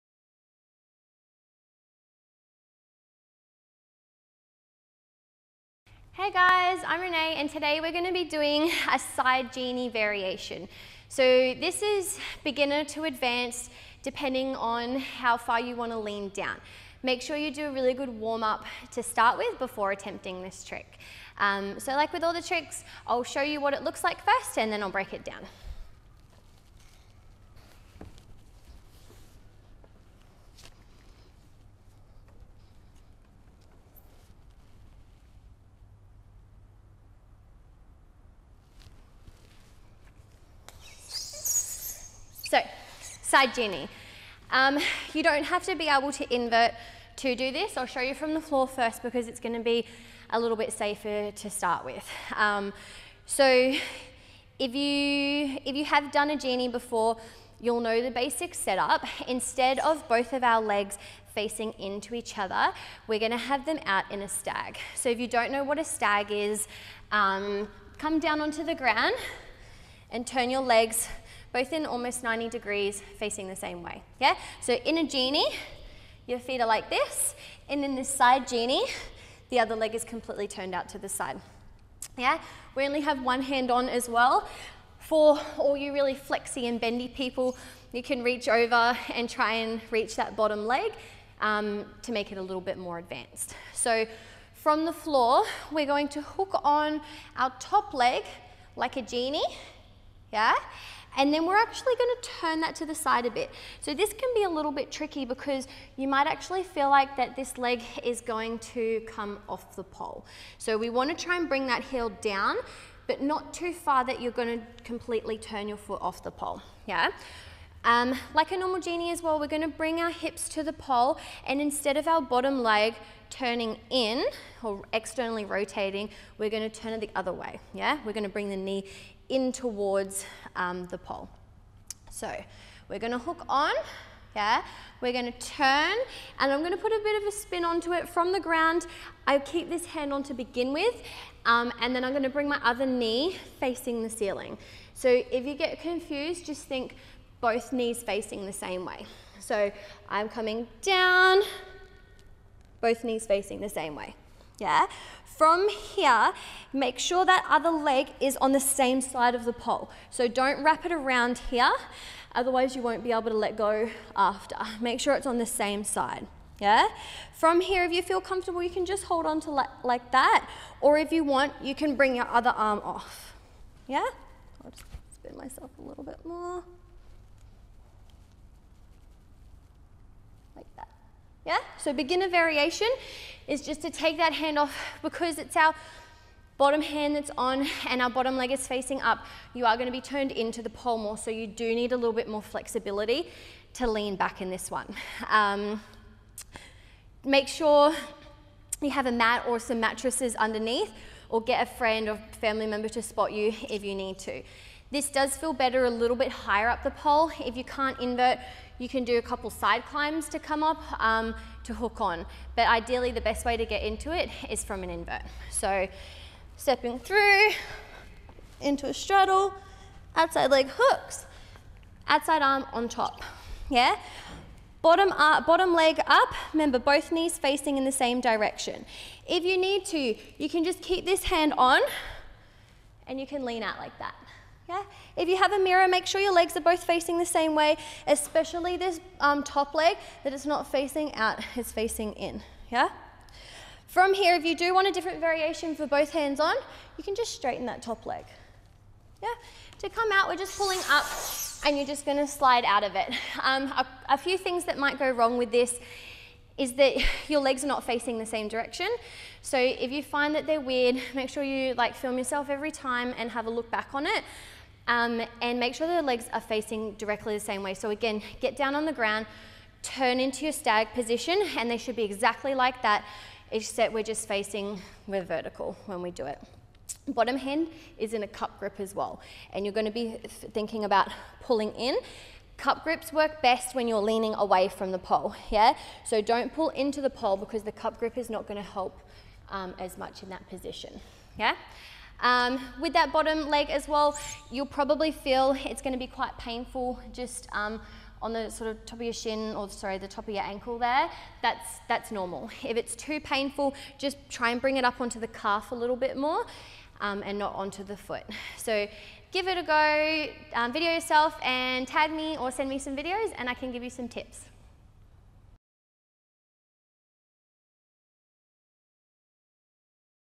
All Levels Pole Dancing Tutorial- Side Genie Variation
Learn this side genie variation with Renee.

This is suitable for ALL LEVELS of Pole Dancers.